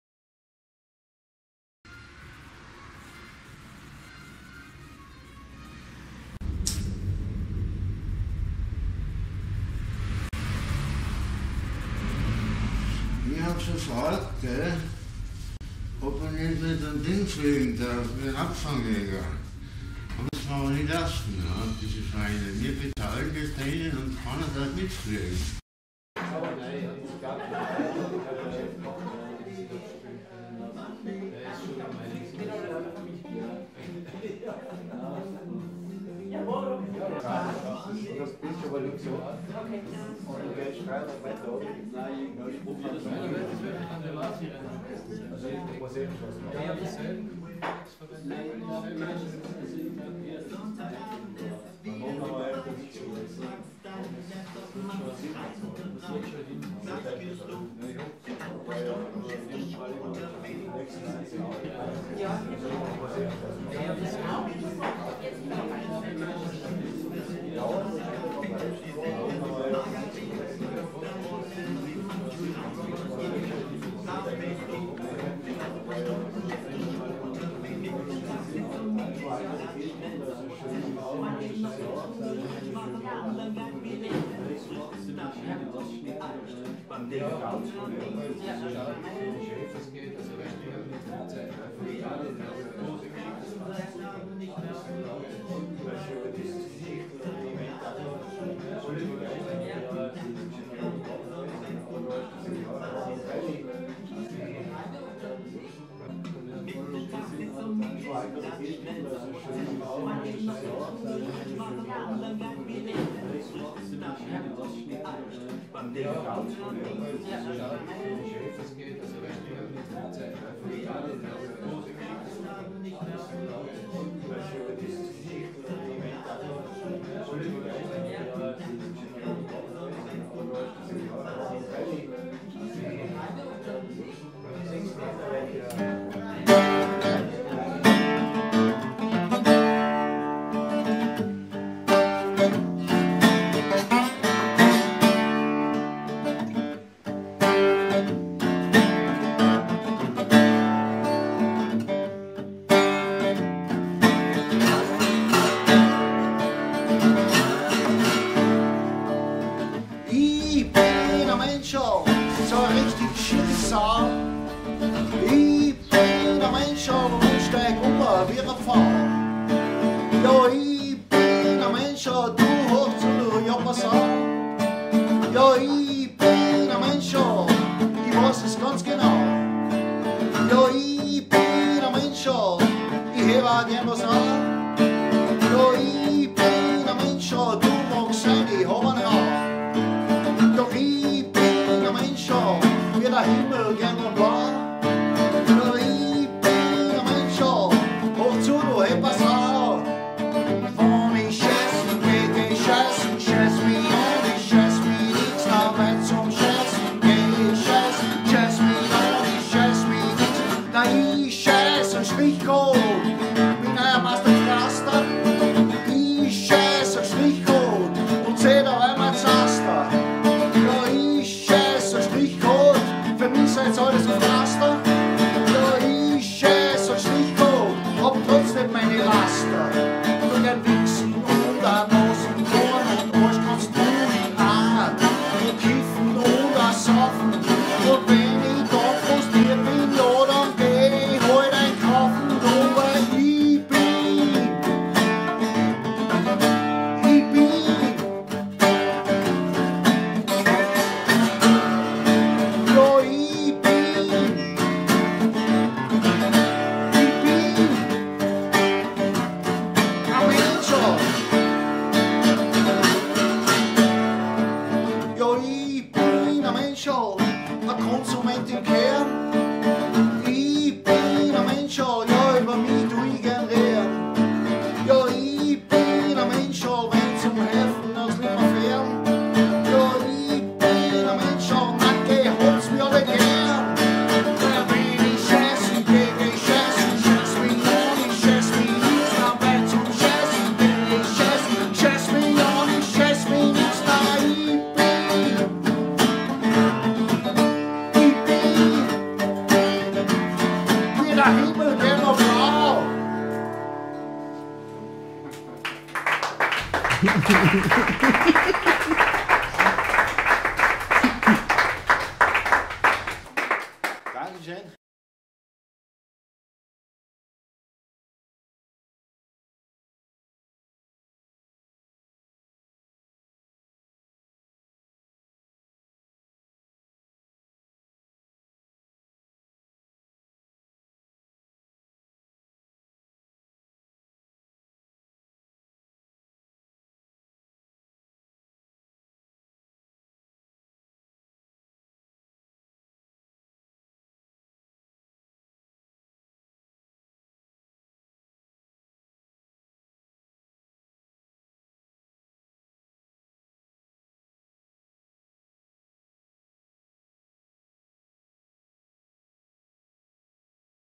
back from universe - Lang Michael
Anlässlich der Ausstellung „back from universe“ des Innsbrucker Malers „Michael Lang“  spielte Soed broed, Songs aus seinem Buch „Lyrische Lazi Trilette in 4 Bänden“ auf dessen Finissage im Kooio in Innsbruck.
Der 1954 geborene Maler, der an der  Hochschule für angewandte Kunst Wien bei Prof. Oberhuber sein Handwerk erlernte, zog es  nach dem Studium erst mal in die Welt. Nebst Reisen nach Indien und Nepal bereiste er auch viele Europäische Länder.